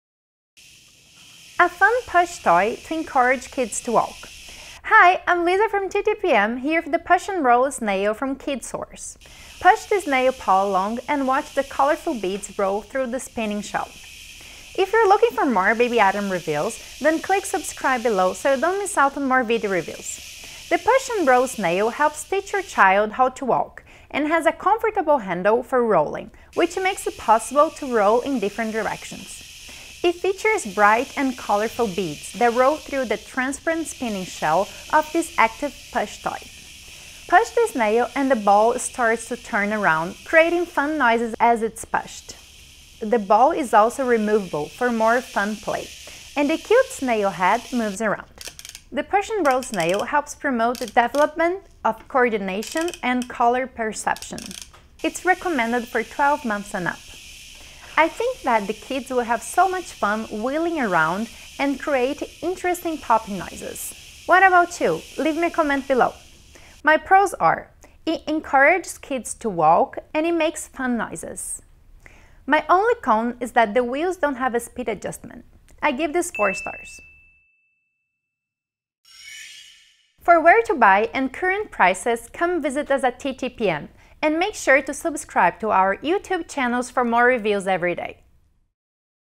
Push N' Roll Snail Infant toy review from KidSource
Today TTPM is reviewing Push N' Roll Snail from KidSource, a fun push-toy to encourage kids to walk. See more in this video review! For full review and shopping info

Product Info: Push this snail pal along and watch the colorful beads roll through the spinning shell. The Push N' Roll Snail helps teach your child how to walk and has a comfortable handle for rolling, which makes it possible to roll in different directions.